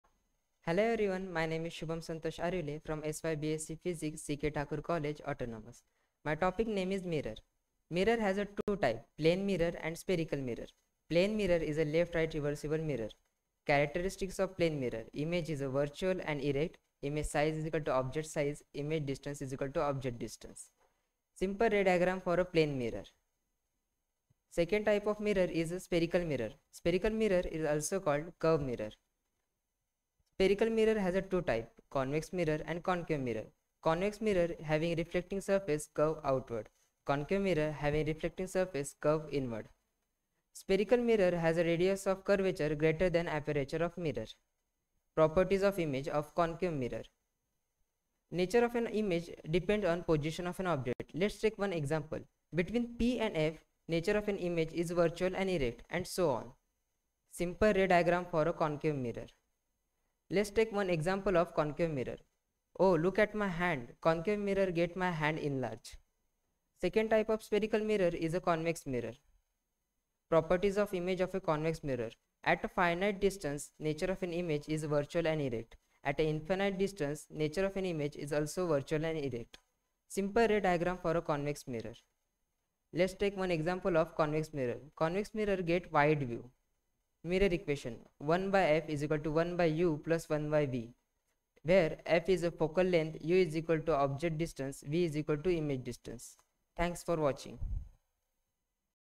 Shubham Arivale, SYBSC-Physics
We proudly acknowledge and celebrate the exceptional achievement of Mr. Shubham Arivale, a remarkable student from SYBSC-Physics at CKT College in Panvel. His extraordinary presentation has been recognized as the best at the national level.

This achievement not only reflects Mr. Shubham Arivale individual brilliance but also highlights the commitment of CKT College in fostering a conducive environment for academic excellence and innovation. The availability of cutting-edge resources like the e-Content Development facility has empowered students to explore new avenues of learning and present their ideas with utmost proficiency.
His outstanding presentation serves as an inspiration to aspiring students and a testament to the transformative capabilities of the e-Content Development facility at CKT College's Library Maker Space.